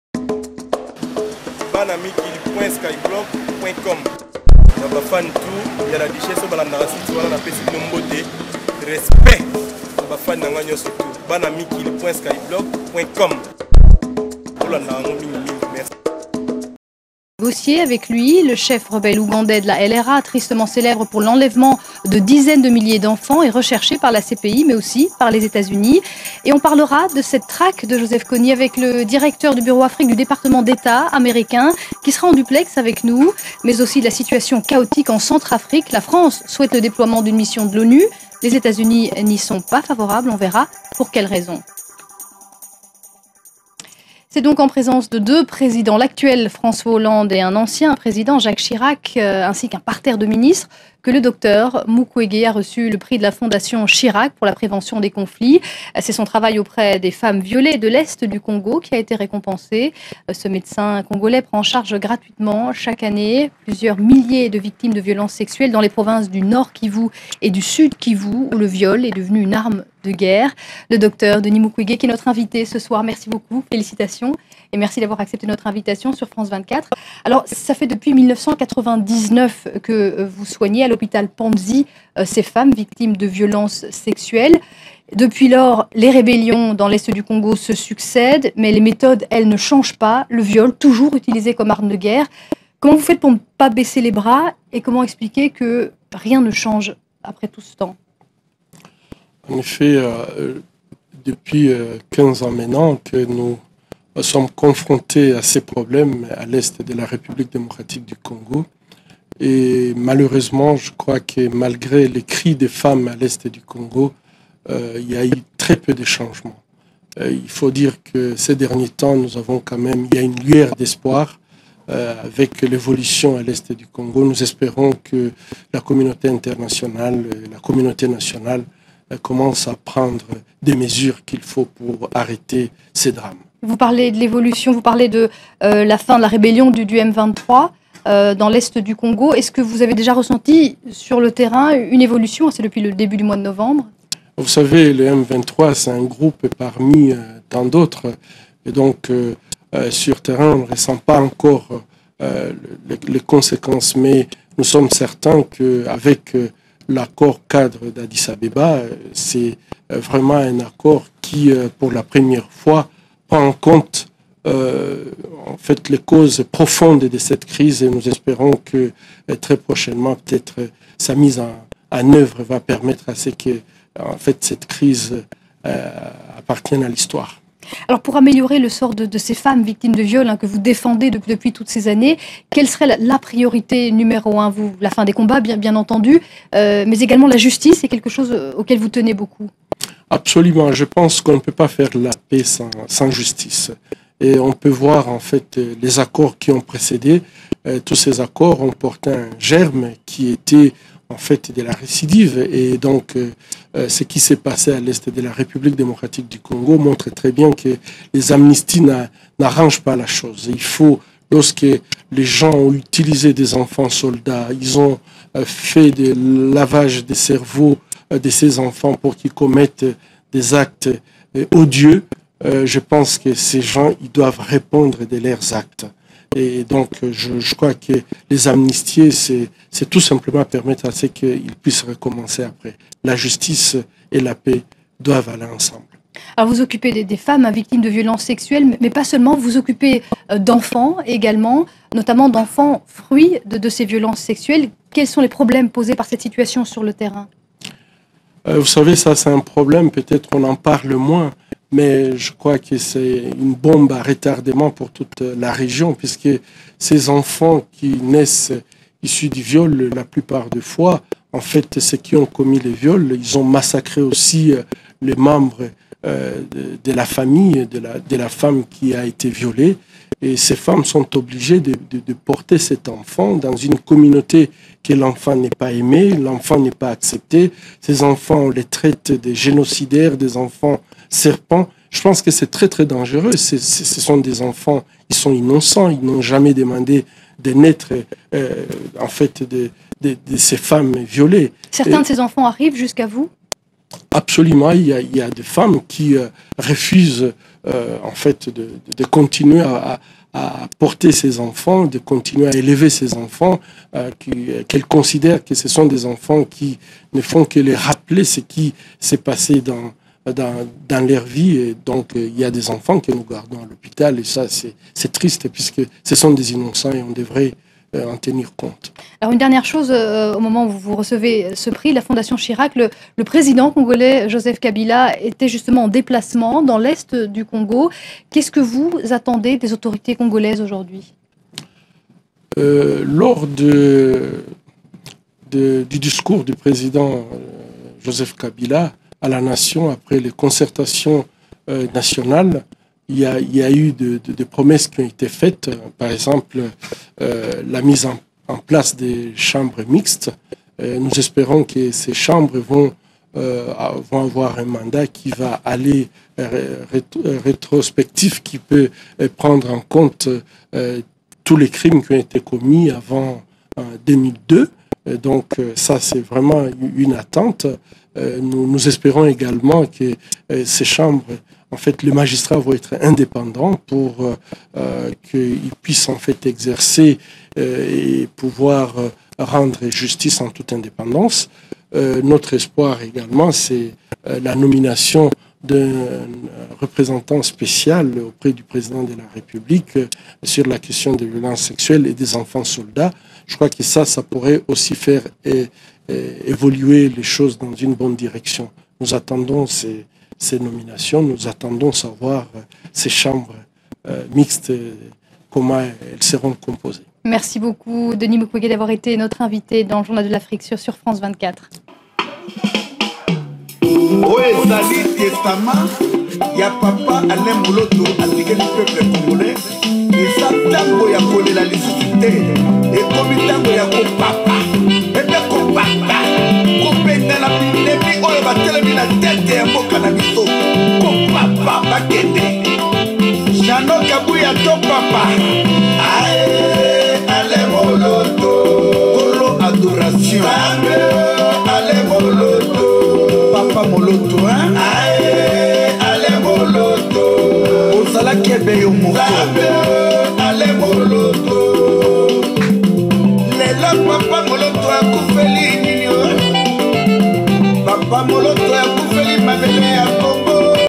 Intervention lucide du Dr Denis Mukwege qui explique pourquoi le M23 doit pas être amnestié
Le docteur congolais Denis Mukwege récompensé pour son travail auprès des femmes violées

L'infatigable défenseur des femmes violées de l'Est du Congo, le docteur Denis Mukwege, récompensé par la Fondation Chirac, est notre invité. Chaque année, le gynécologue congolais soigne gratuitement plus de 3 500 femmes victimes de viols, véritables armes de guerre en RDC.